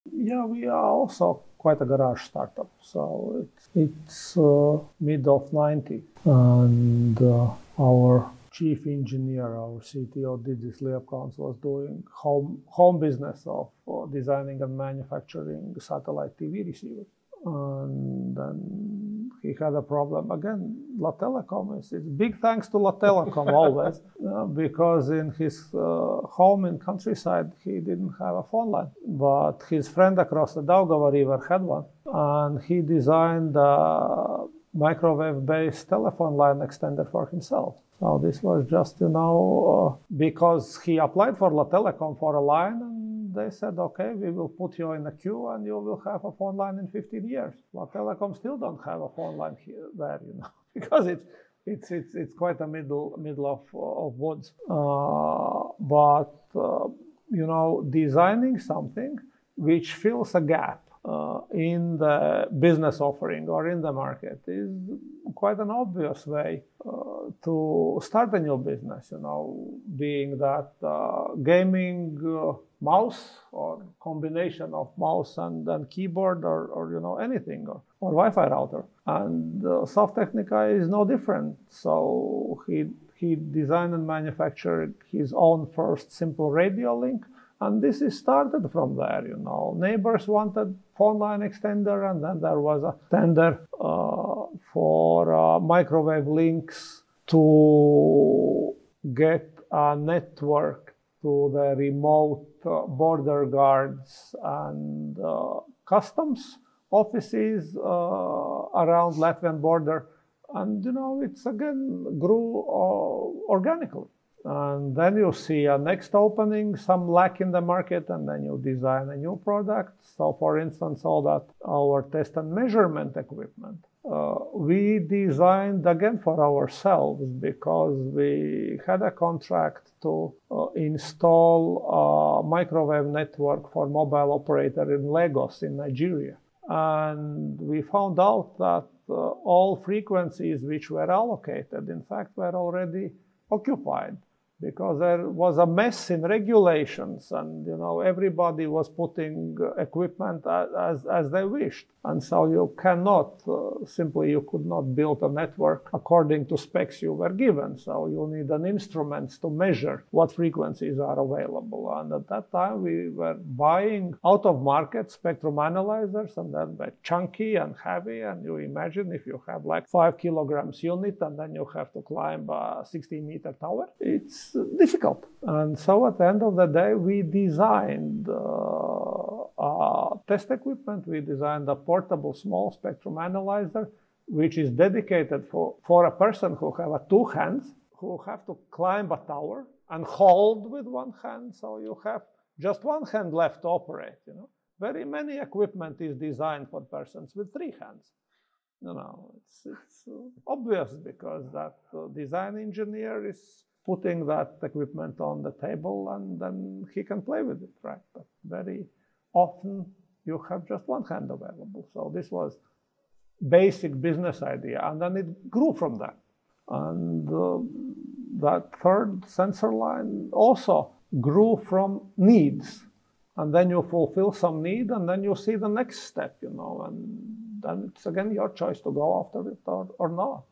How did SAF Tehnika start?
Answer by Normunds Bergs, CEO of SAF Tehnika.